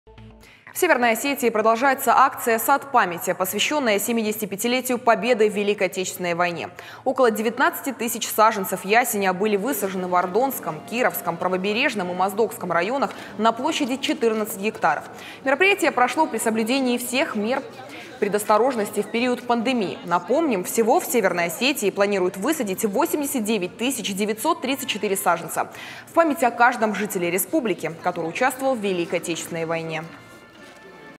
В Северной Осетии продолжается акция «Сад памяти», посвящённая 75 летию Победы в ВОВ
В Северной Осетии продолжается акция «Сад памяти», посвящённая 75-летию Победы в Великой Отечественной войне. Около 19 тысяч саженцев ясеня были высажены в Ардонском, Кировском, Правобережном и Моздокском районах на площади 14 гектаров. Мероприятие прошло при соблюдении всех правил и мер предосторожности в период пандемии.

Напомним, всего в Северной Осетии планируют высадить 89 тысяч 934 саженца - в память о каждом жителе республики, участвовавшем в Великой Отечественной войне.
Мы рядом в:
ok.ru/irystontv